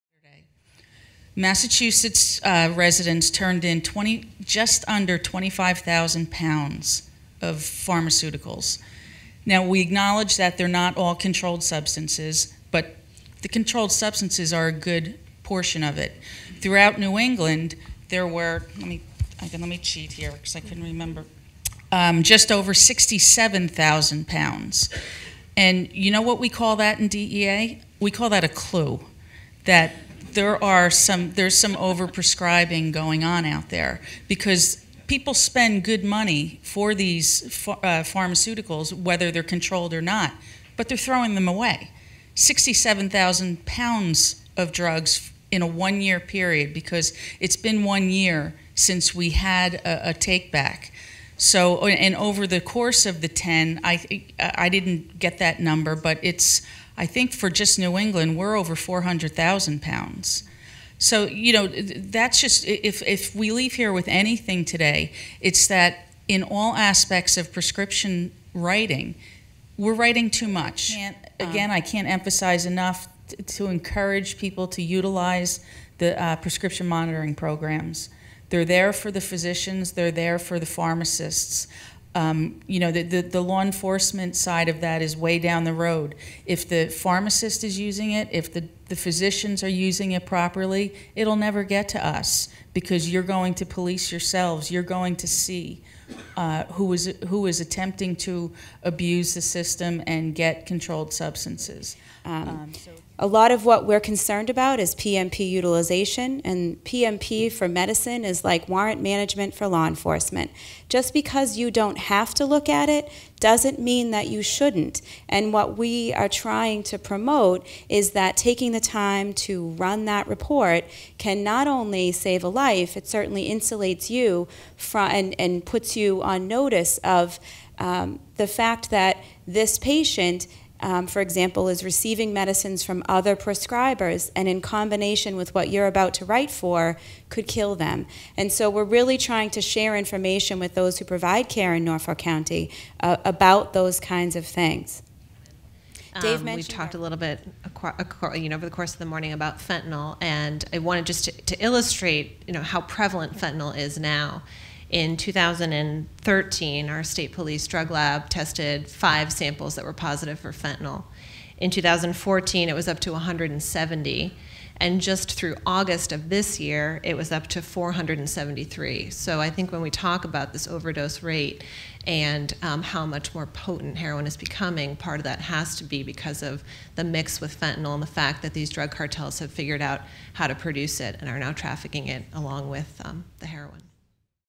The Importance of Prescription Monitoring Programs
At the MMS-US Department of Justice Summit on Opioid Misuse and Addiction, a panel of law enforcement officials promote prescription drug programs as a valuable tool for pharmacists and physicians to combat drug addiction.